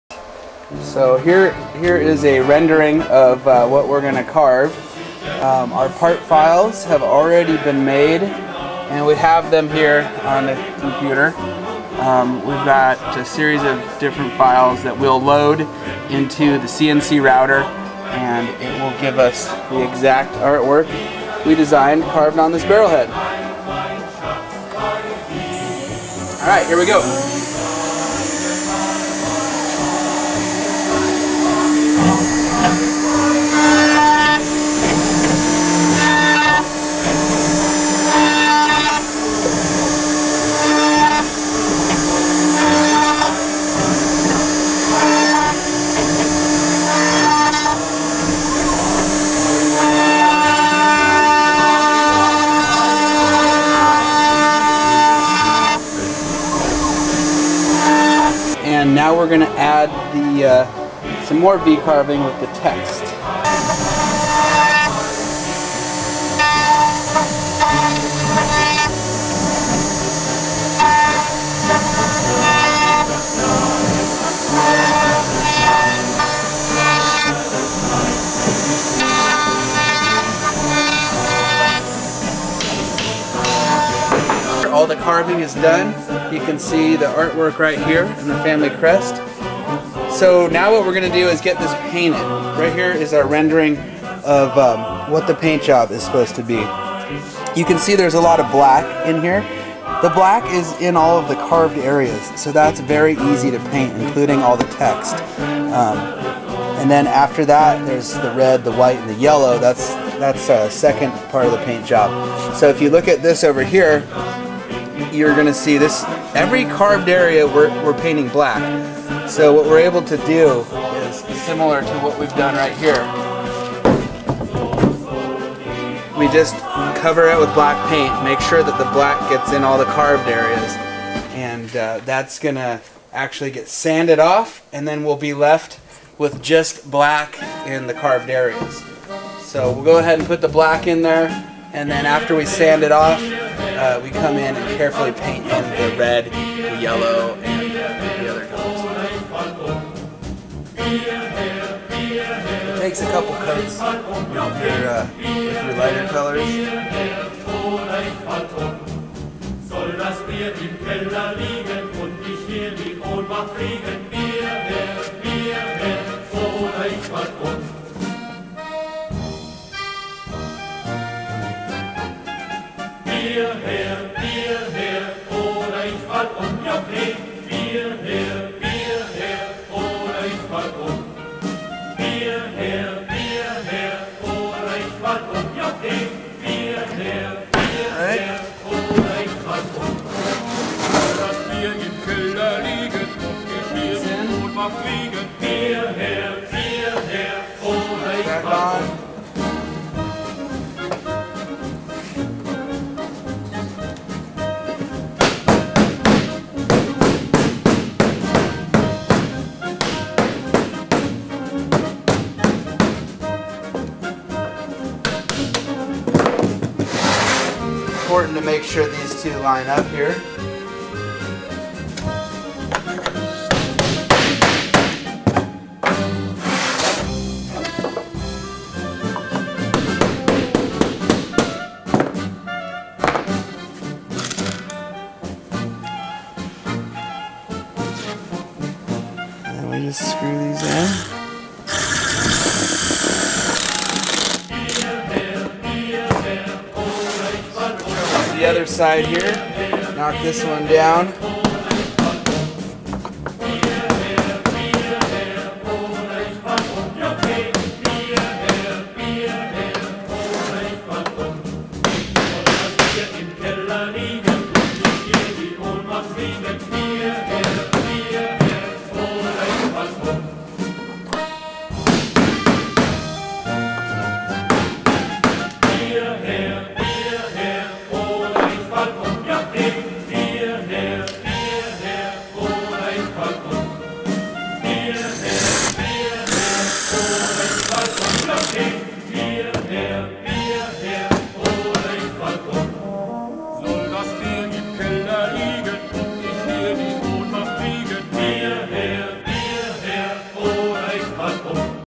How to Carve a Wine Barrel Carving for Oktoberfest
We specialize in custom wine barrel art carvings that can be personalized. In this video we show you how we make a personalized barrel head carving for a family with custom art and text for Oktoberfest.